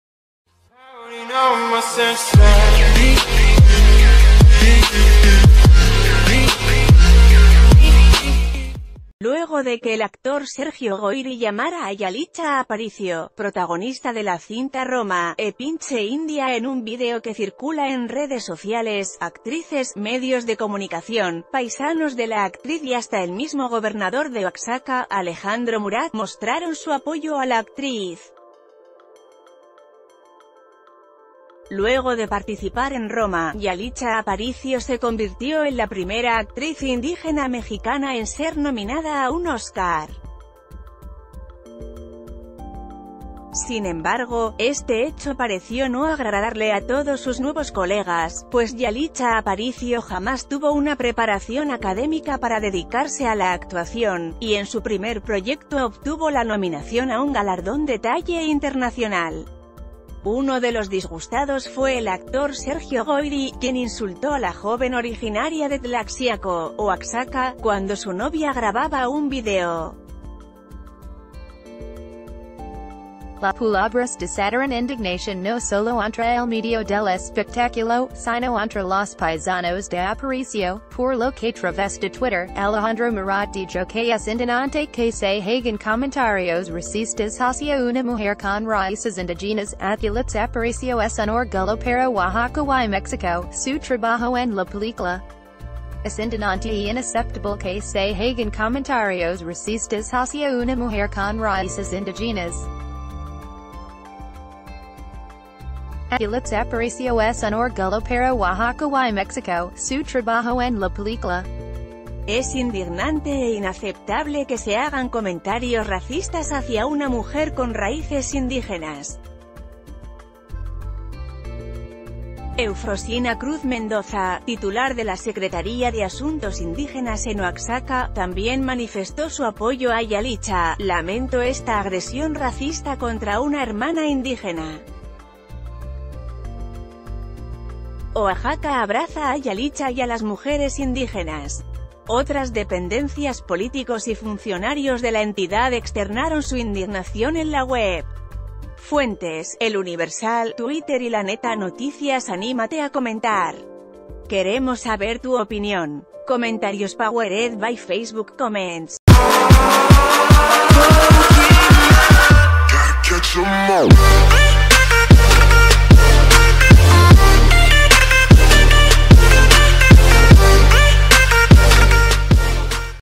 Oaxaca reafirma apoyo a Yalitza Aparicio tras insulto de Sergio Goyri
Luego de que el actor Sergio Goyri llamara a Yalitza Aparicio, protagonista de la cinta Roma, ‘pinche india’ en un video que circula en redes sociales, actrices, medios de comunicación, paisanos de la actriz y hasta el mismo gobernador de Oaxaca, Alejandro Murat, mostraron su apoyo a la actriz. Lueg...